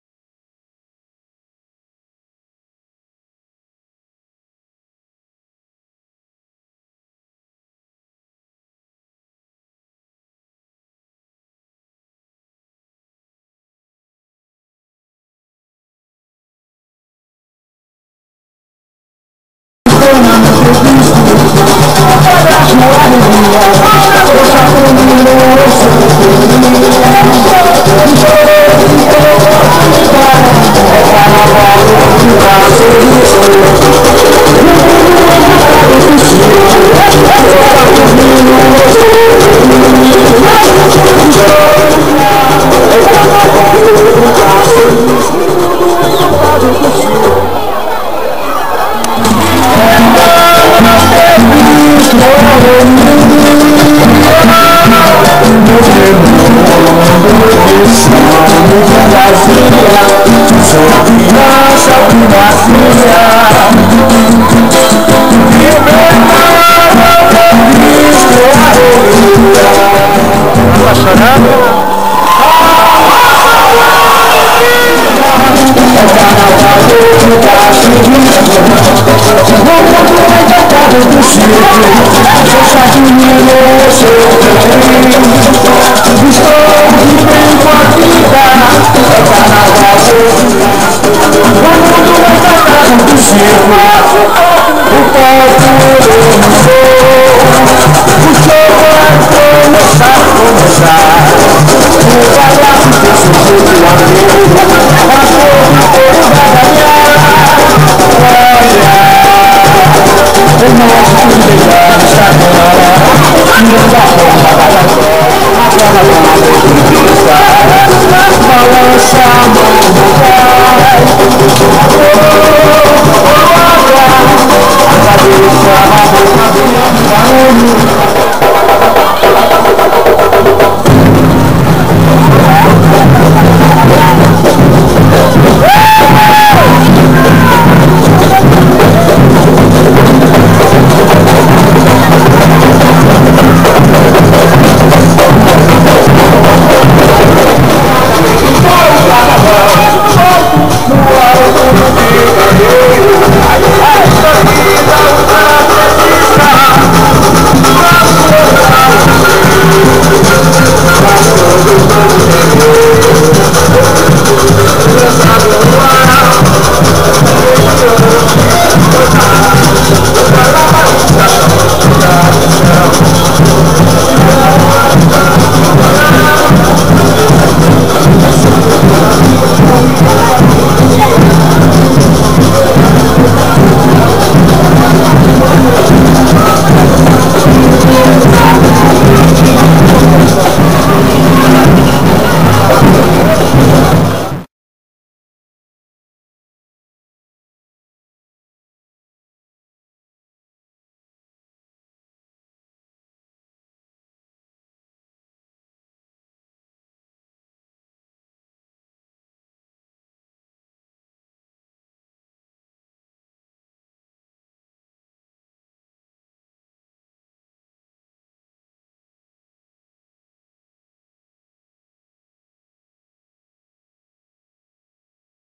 Escola de Samba Saturnino de Brito - Santos/SP
Carnaval 2011 - Dudu e Belinha e seus amiguinhos participando do desfile de carnaval da escolinha Saturnino de Brito, no bairro marapé, em Santos.